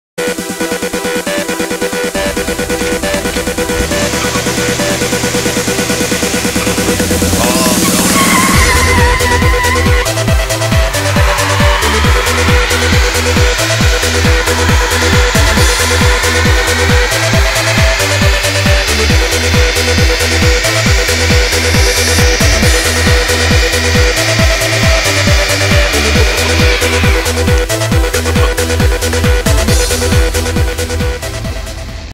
Extreme Van Burnout 2
Me and my cousins doing a burnout in the old van on a county road.